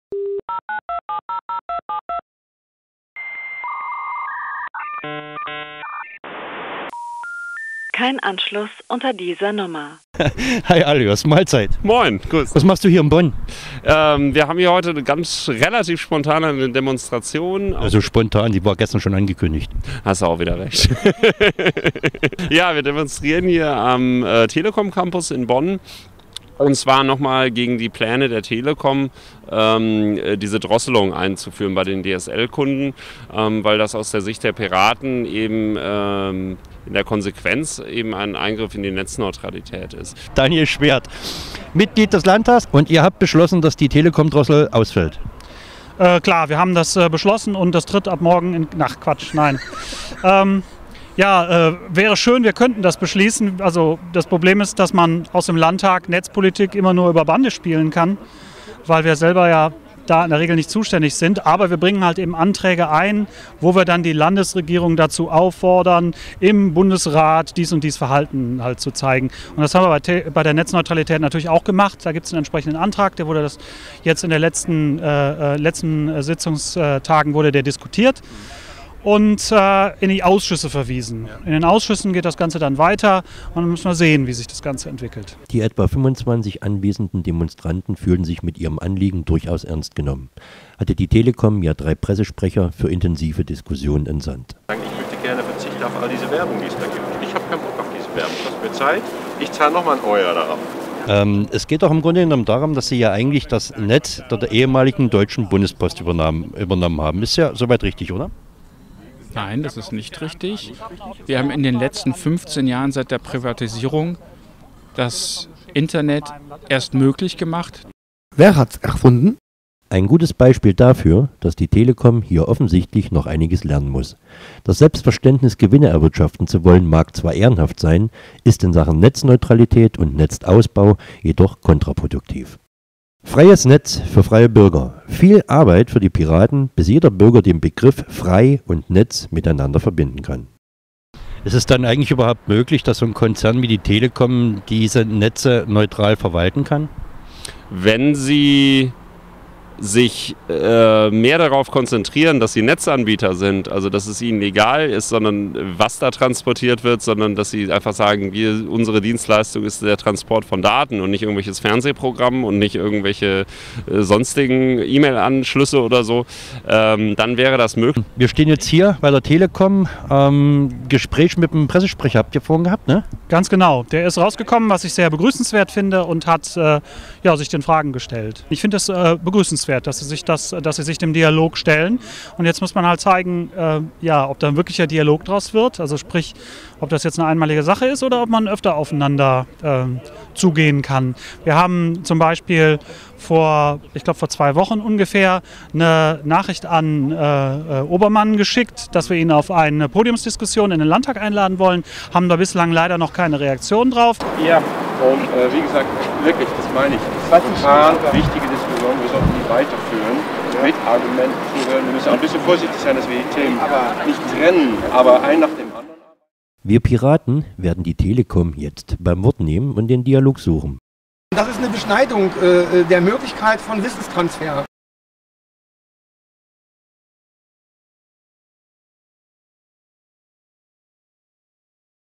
Zusammenfassung Drosselkom Demo in Bonn - 05.06.2013
Zusammenfassung der Demo am Telekom Campus in Bonn vom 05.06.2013.

Mit dabei:
Markus Barenhoff und Daniel Schwerd